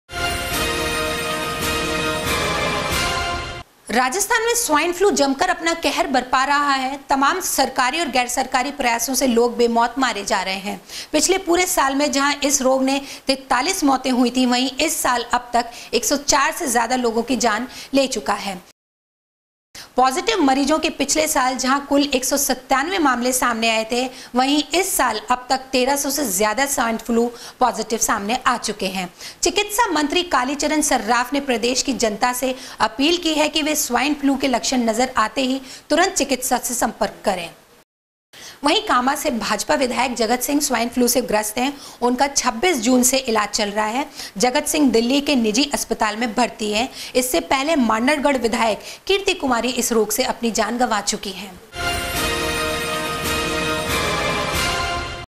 स्वाइन फ्लू ने फिर पसारे पैर
राजस्थान में स्वाइन फ्लू जमकर अपना कहर बरपा रहा है। तमाम सरकारी और गैरसरकारी प्रयासों के लोग बेमौत मारे जा रहे हैं। पिछले पूरे साल में जहां इस रोग से 43 मौतें हुई थी वहीं इस साल अब तक 104 से ज्यादा लोगों की जान ये रोग ले चुका है। पॉजिटिव मरीजों के पिछले साल जहां कुल 197 मामले सामने आए थे। वहीं इस साल अत तक 1300 से ज्यादा स्वाइन फ्लू पॉजिटिव सामने आ चुके है।
वहीं, कामां से भाजपा विधायक जगतसिंह स्वाइन फ्लू से ग्रस्त हैं। उनका 26 जून से इलाज चल रहा है। जगत सिंह दिल्ली के निजी अस्पताल में भर्ती हैं। इससे पहले मांडलगढ़ विधायक कीर्ति कुमारी इस रोग से अपनी जान गंवा चुकी हैं।